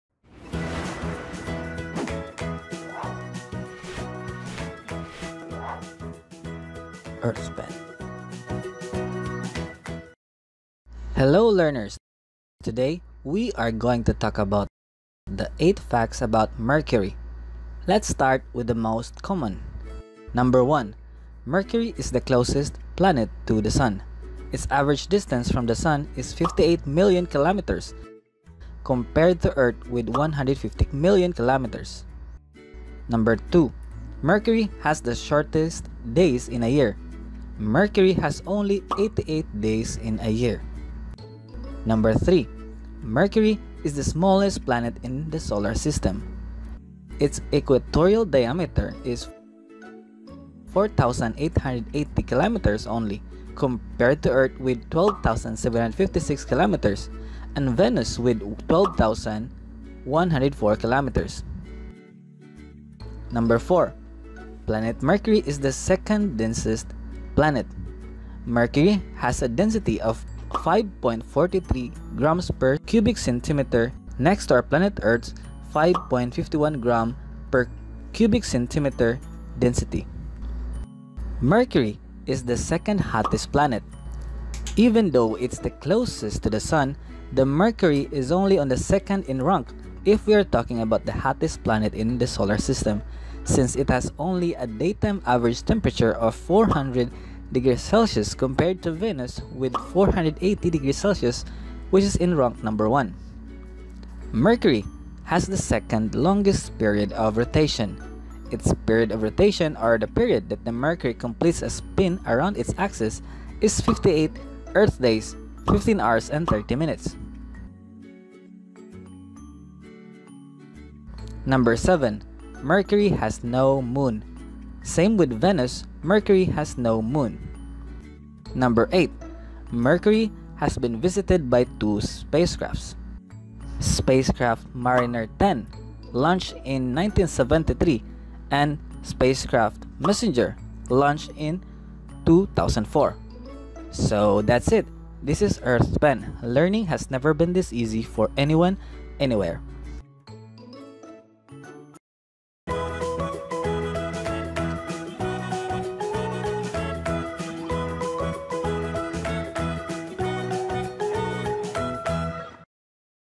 8 Facts You Need to Know About Mercury | Animation
This video explains "8 Facts You Need to Know About Mercury" in a fun and easy way.